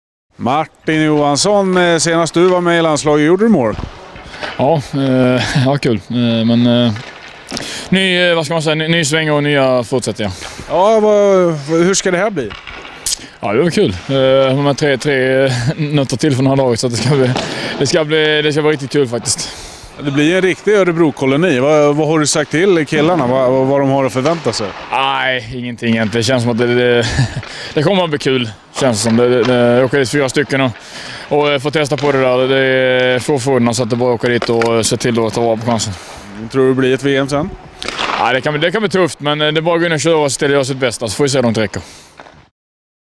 Martin Johansson: ”Det ska bli kul, det är ju tre nötter till från det här laget”
Martin Johansson om att återigen bli uttagen till Tre Kronor.